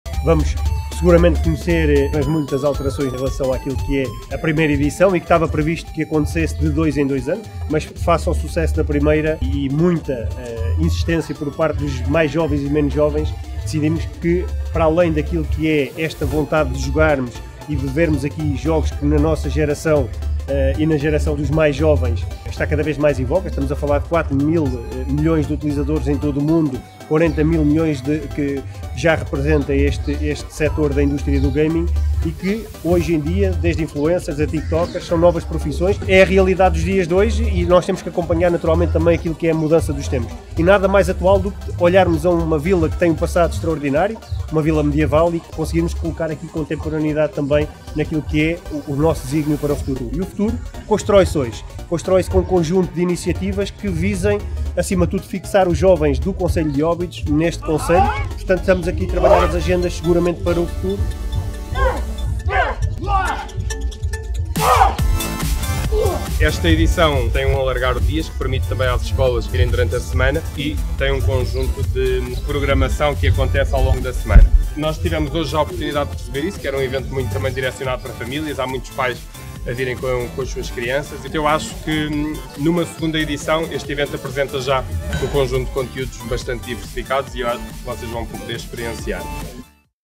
Inauguração Óbidos Vila Gaming 2024
Inauguração Óbidos Vila Gaming 2024, no dia 4 de Abril,
a partir do posto de turismo até à cerca do castelo.

informações em obidos.pt